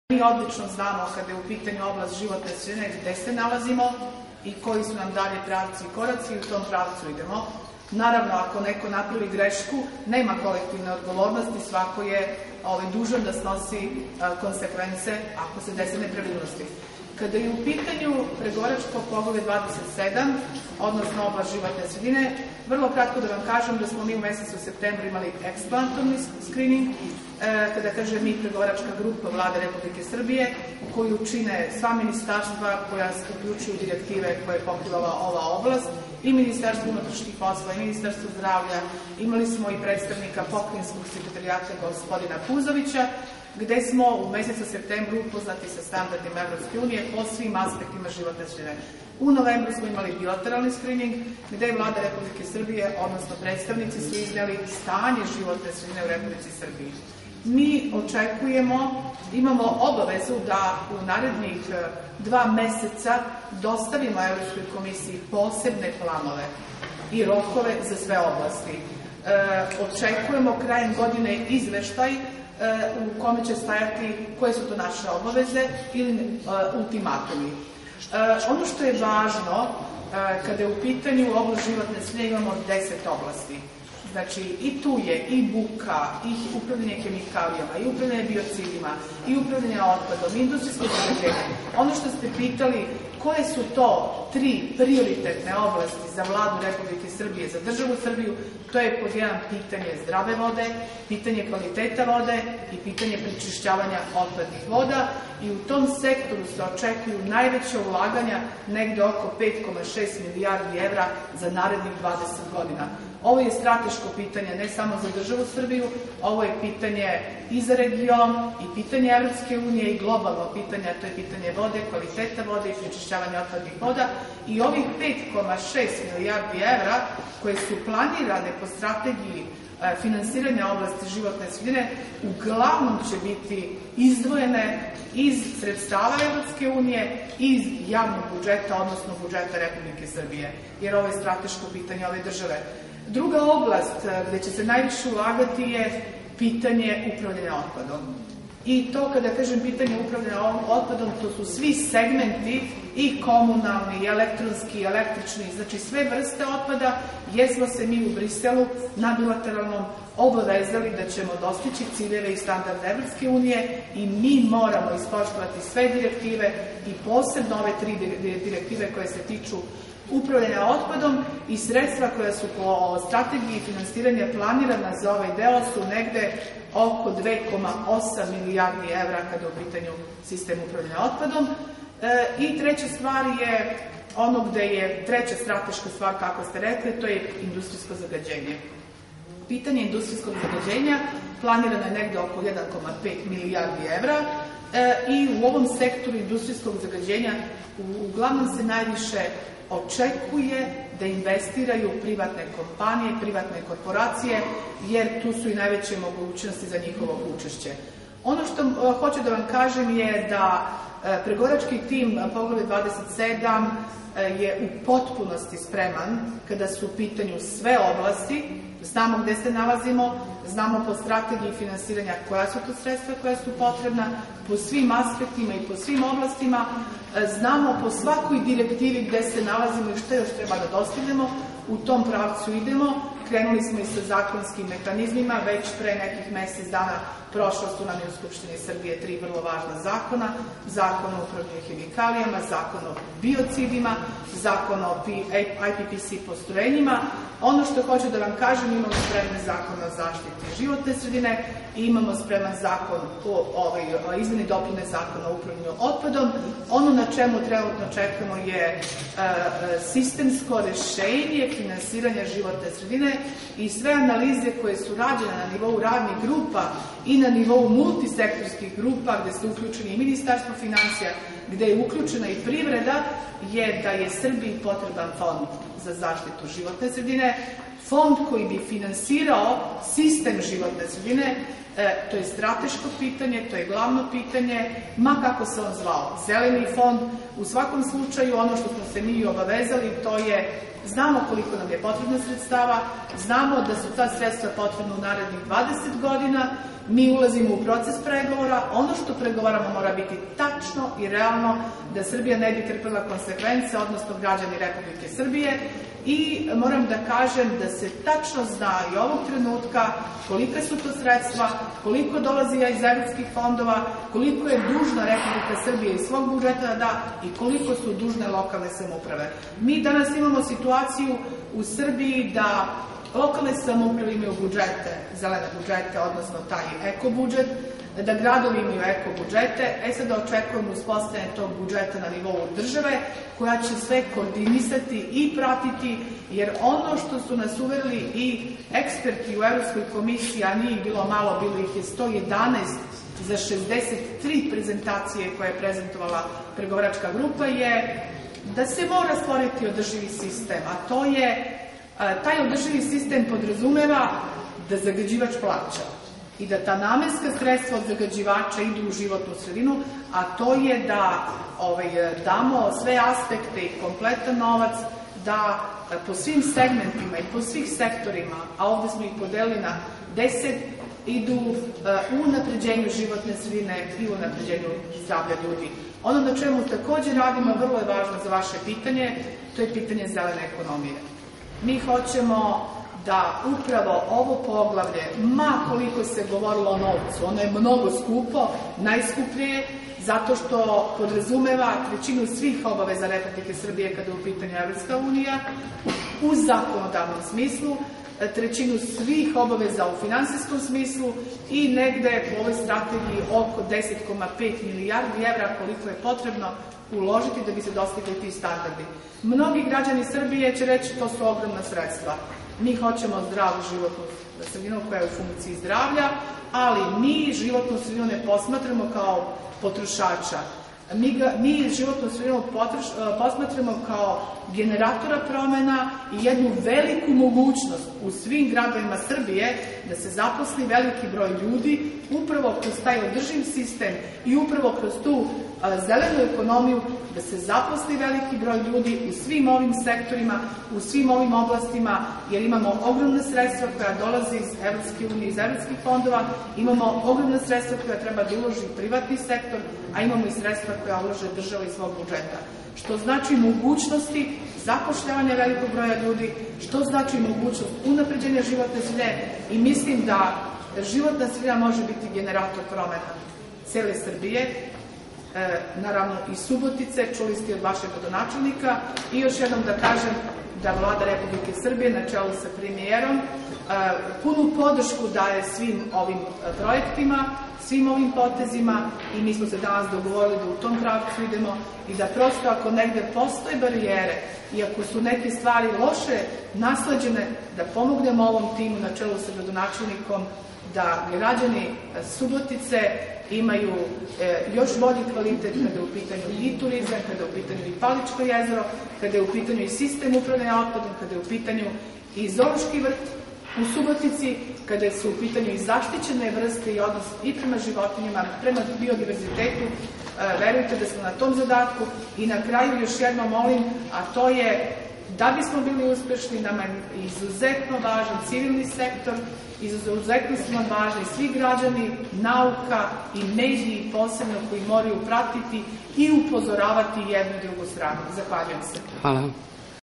Stana Bozovic zivotnu sredinu posmatramo kao generatora promena
super-info.rs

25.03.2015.

Stana Božović: Životnu sredinu posmatramo kao generatora promena

U okviru Pregovaračkog poglavlja 27, koje se odnosi na oblast zaštite životne sredine, definisano je
ukupno deset oblasti od kojih su najznačajnije i prioritetne oblasti: pitanje zdrave pijaće vode, kvaliteta vode i prečišćavanja otpadnih voda, gde se po rečima državne sekretarke u Ministarstvu poljoprivrede i zaštite životne sredine Stane Božović, očekuju najviša ulaganja od oko 5,6 milijardi evra za narednih 20 godina.

- Ovo je strateško pitanje, ne samo za Srbiju, već za region, EU i globalni nivo, rekla je Božović i kao oblast od prioritetnog značaja, gde su takođe planirana značajna ulaganja od 2,8 milijardi evra, navela segment upravljanja otpadom.

Među strateškim ciljevima je i uređenje oblasti industrijskog zagađenja gde su planirana ulaganja od oko 1,5 milijardi evra, a na ovom planu se najviše očekuju investicije od strane privatnih kompanija.

- Na iznalaženju sistemskog rešenja za finansiranje oblasti zaštite životne sredine, sve analize koje su rađene na nivou radnih grupa, multisektorskih grupa gde su uključeni Ministarstvo finansija i privreda, upućuju na zaključak da je Srbiji potreban Fond za zaštitu životne sredine, koji bi finansirao ovu oblast, rekla je Božović.

Ona je takođe istakla neophodnost, na koju su ukazali i brojni ekspertski timovi iz EU.
- Mora se izgraditi održivi sistem, koji podrazumeva da zagađivač plaća, a da ta sredstva finansiraju oblast unapređenja životne sredine i zdravlja ljudi. Od posebnog značaja je i pitanje zelene ekonomije.

Za Poglavlje 27, Božović kaže da je ono najkompleksnije i zahteva najviše novca.
- Mi hoćemo da ovo poglavlje, koje je mnogo skupo, najskuplje, jer podrazumeva većinu obaveza Repubike Srbije kada je u pitanju EU, u zakonodavnom smislu. Zatim, trećinu svih obaveza u finansijskom smislu. To su ogromna sredstva, ali mi hoćemo zdravu životnu sredinu, ali je ne posmatramo kao potrošača, već kao generatora promena i veliku mogućnost u svim gradovima Srbije za zapošljavanje velikog broja ljudi, upravo kroz održivi sistem i zelenu ekonomiju, kaže Božović.